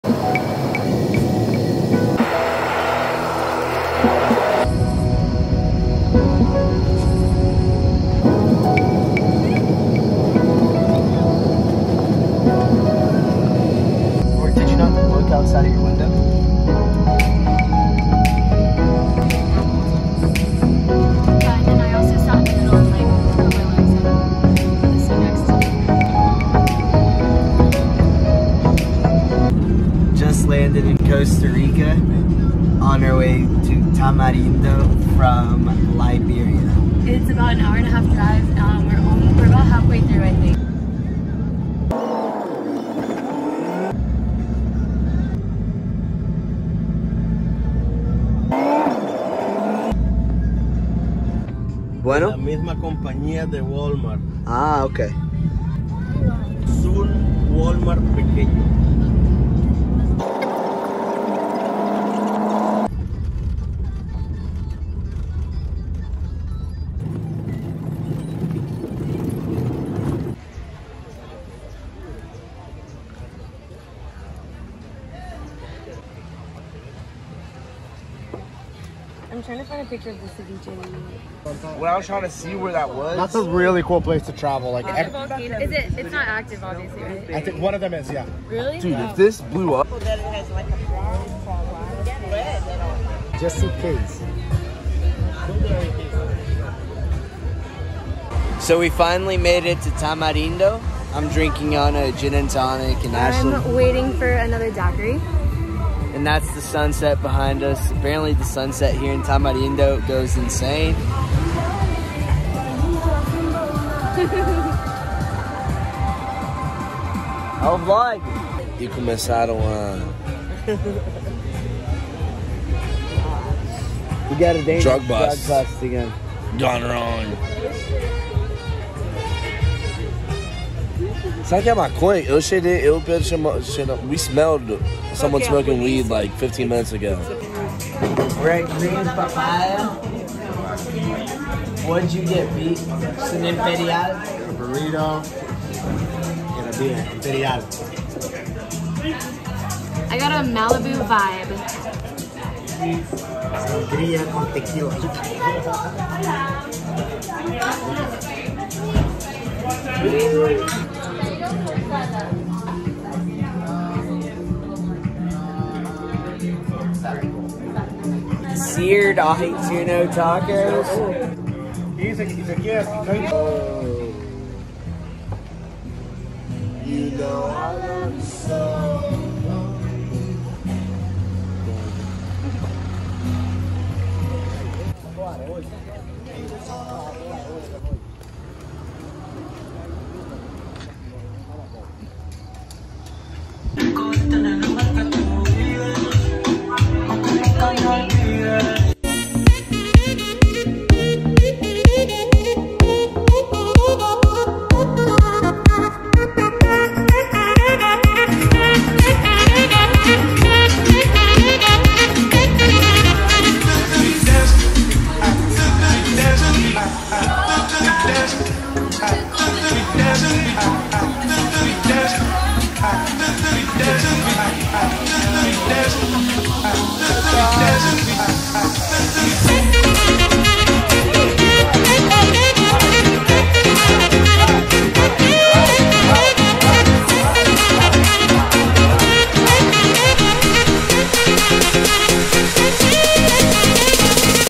WILD NIGHT IN TAMARINDO COSTA RICA (VLOG 1)
Take a journey with us on our trip to the amazing TAMARINDO, COSTA RICA. From beautiful beaches to delicious drinks to sensational surfing, Tamarindo has it all. Follow along as we show you some of the best bars, beaches, and booze in all of Costa Rica. On this trip, we spent all day on the beach until the sky revealed the beautiful sunset across the ocean's horizon. Watch as we laugh, dance, and make memories with our loved ones. If you liked our Saint Martin video, you'll be sure to love this one even more.